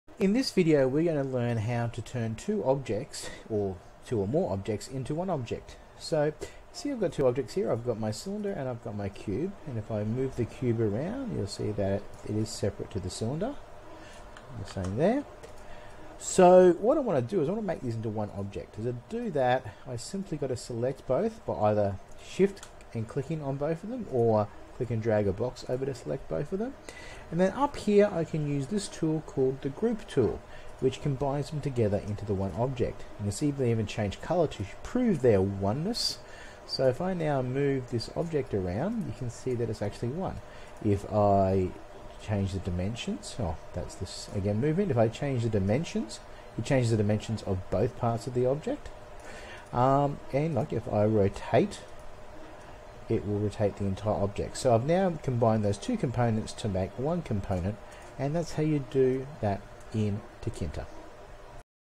Tinkercad - combining objects
A short video on how to combine multiple objects to create one object in Tinkercad.